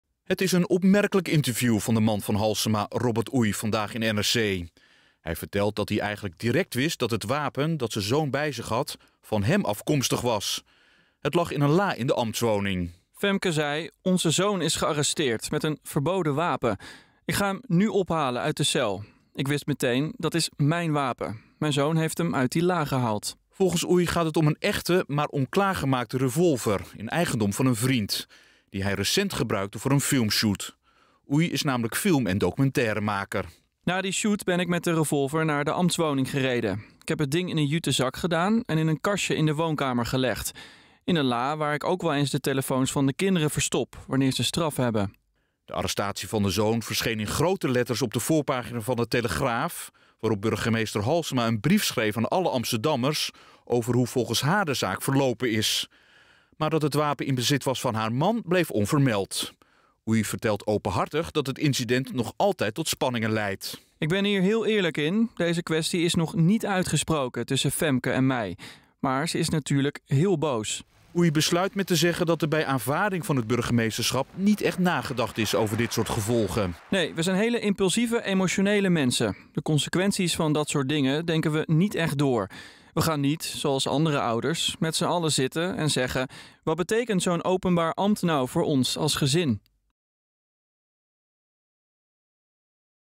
Zoon Halsema nam wapen van vader mee uit ambtswoning
Het verboden nepwapen dat de zoon van burgemeester Halsema in juli bij zich droeg, was van zijn vader. Het was een echte revolver, die onbruikbaar is gemaakt en bij het draaien van films wordt gebruikt.